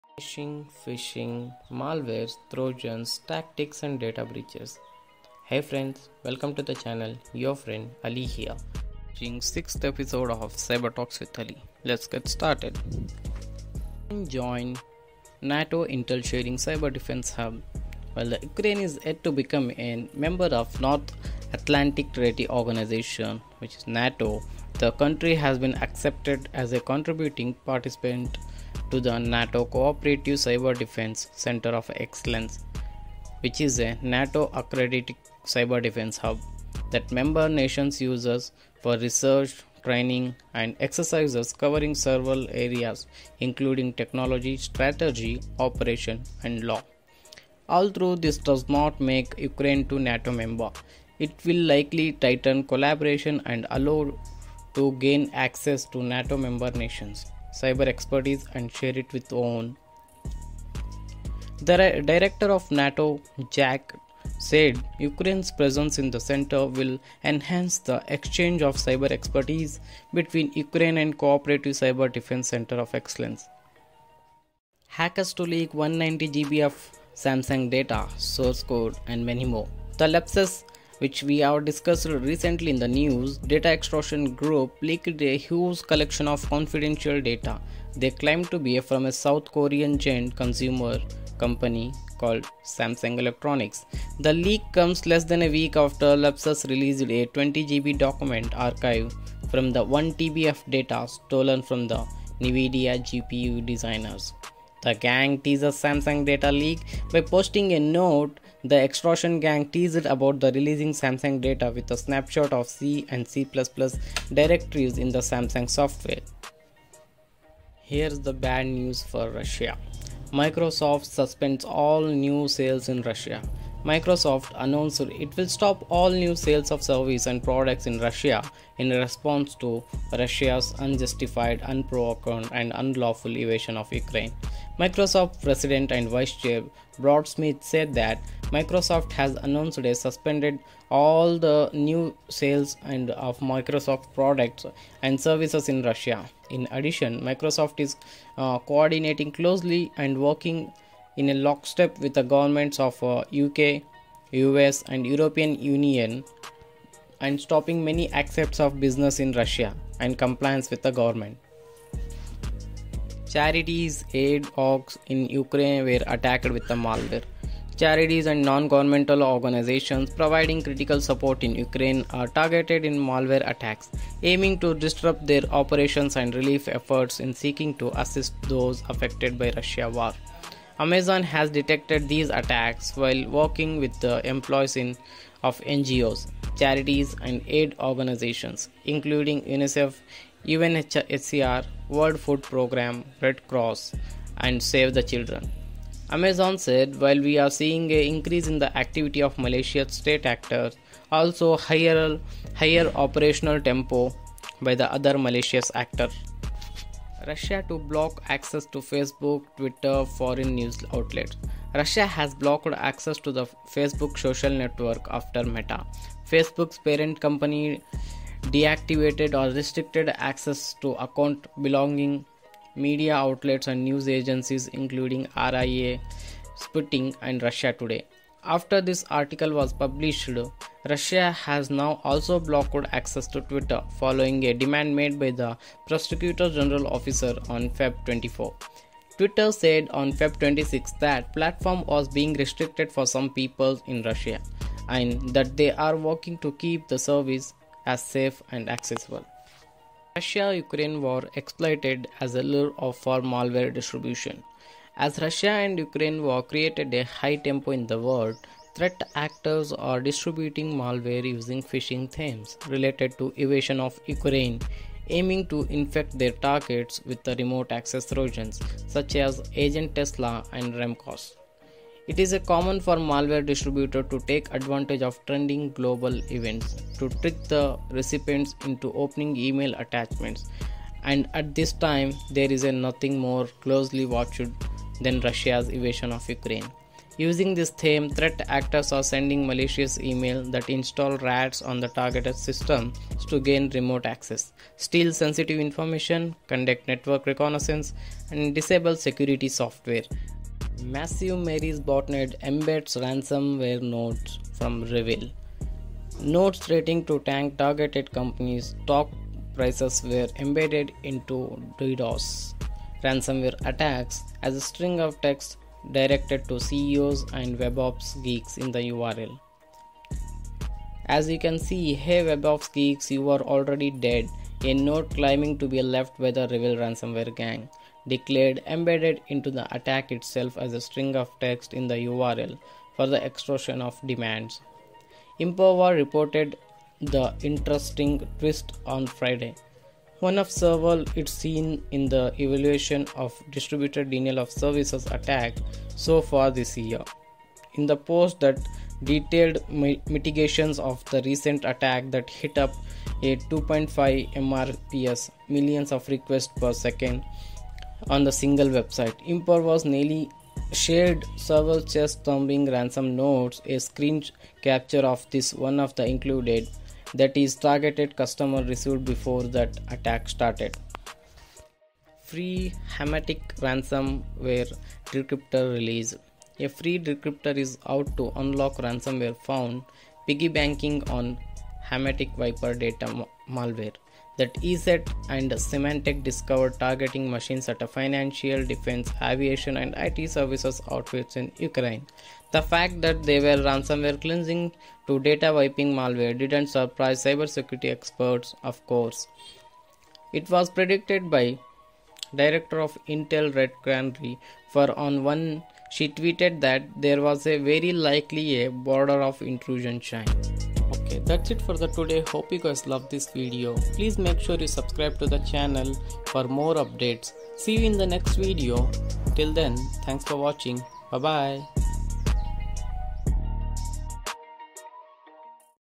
Cyber Security News | 4th and 5th March 2022
Hey Everyone,
In this video, I have discussed news related to cyber security.